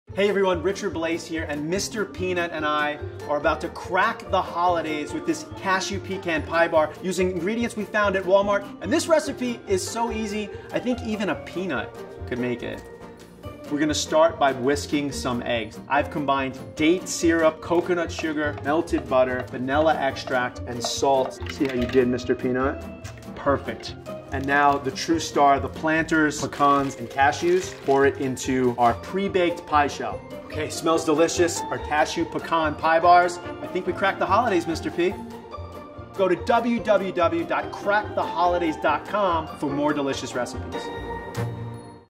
Planters | Mr. Peanut & Richard Blais’ Cashew Pecan Pie Bars
Spice up your holiday menu with this delicious recipe for Cashew Pecan Pie Bars crafted specially by Mr. Peanut and renowned chef Richard Blais.